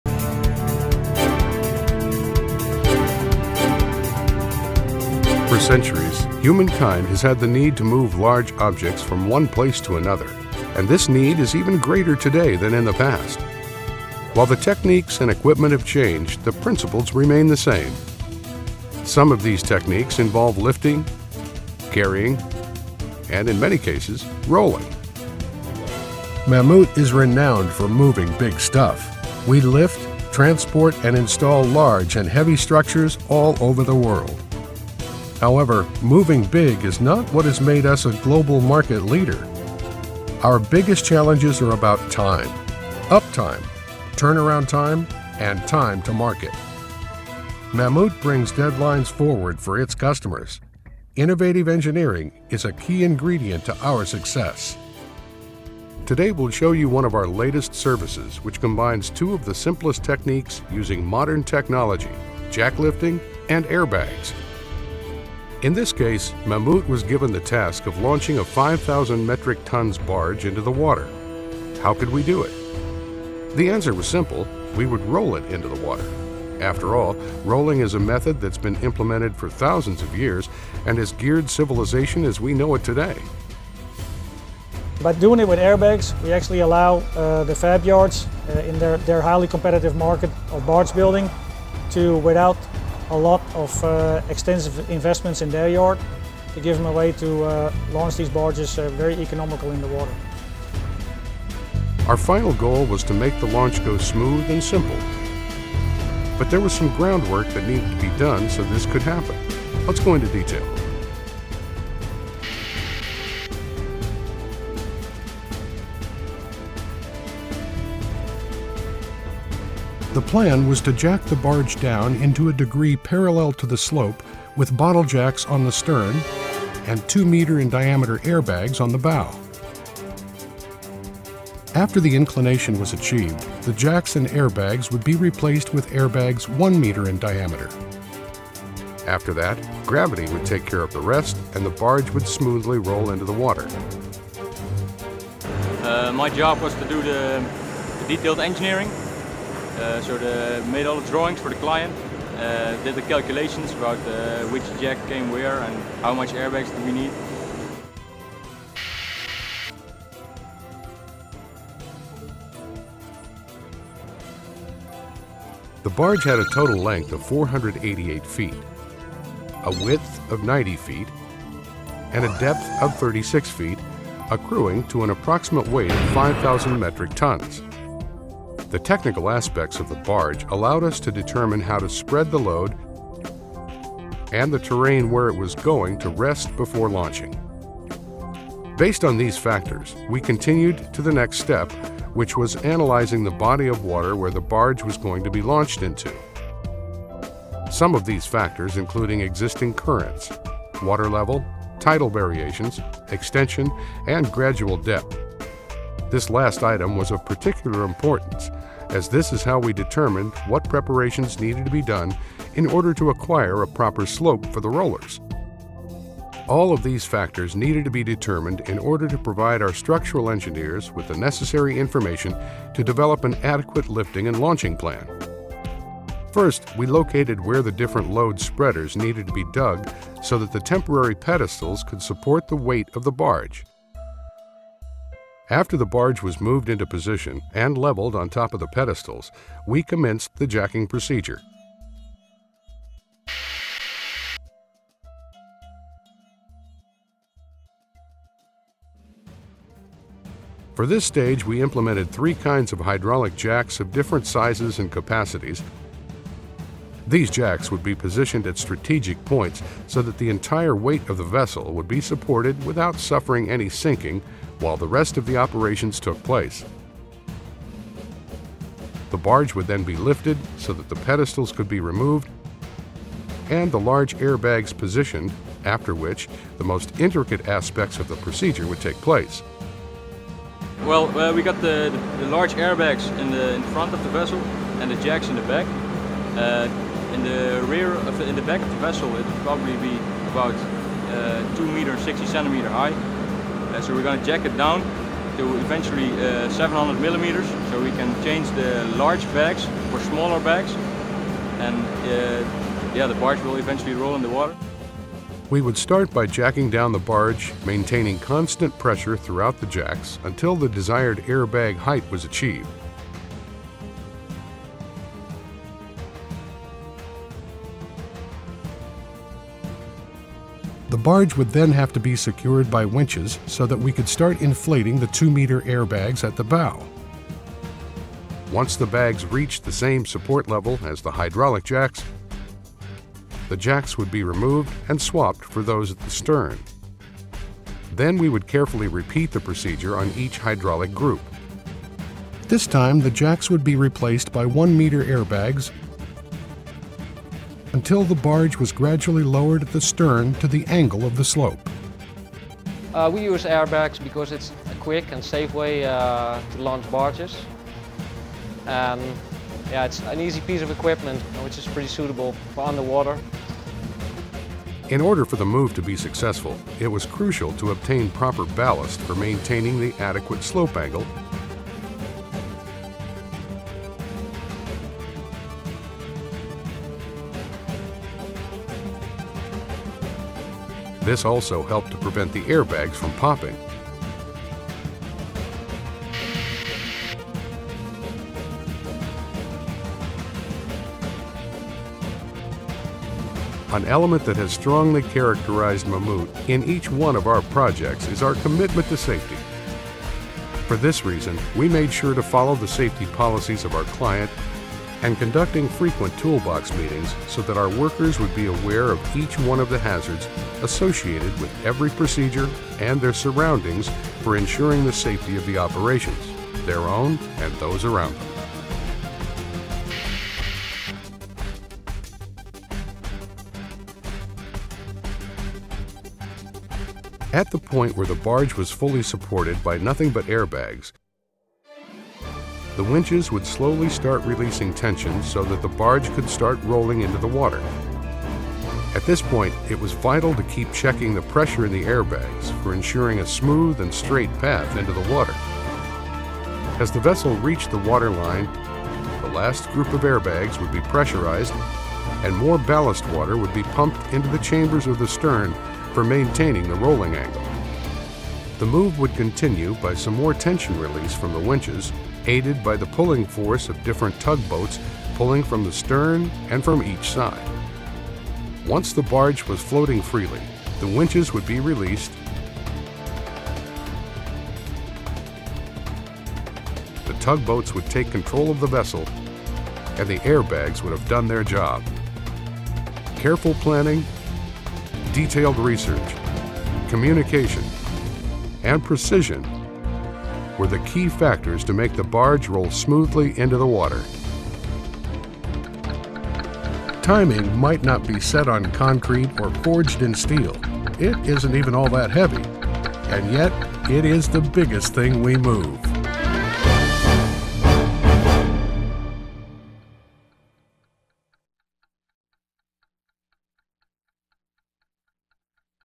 Mammoet - Airbag launch of a 5000 ton barge in USA
Video of a launch of a 5000 ton barge using the inflatable roller bag system